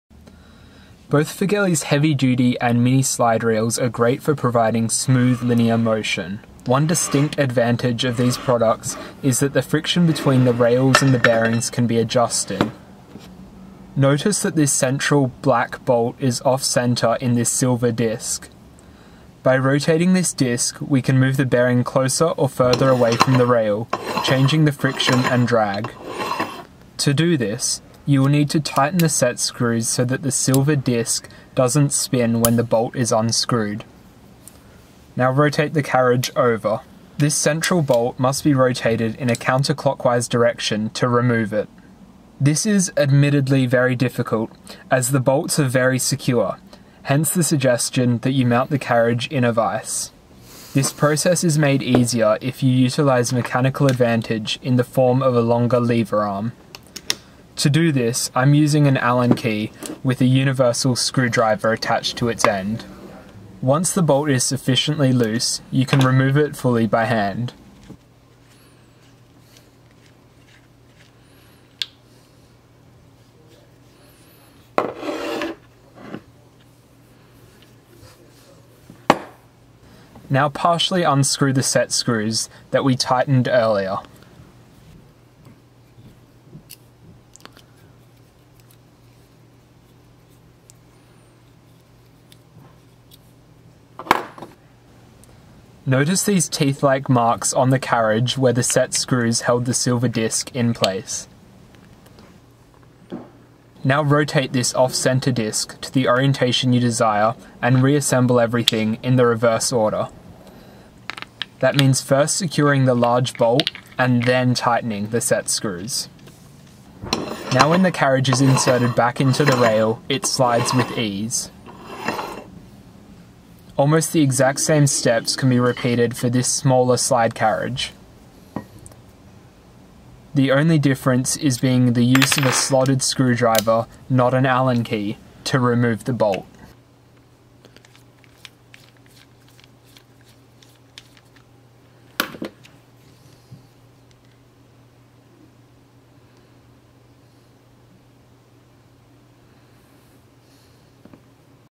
Slide Rail Friction Adjustment Firgelli Automations (FA-SGR-35 & FA-SGR-15N) - How Slide Rails work
Tutorial on how to adjust the central bearing on Firgelli Automations heavy duty (FA-SGR-35) and mini linear track rail (FA-SGR-15N).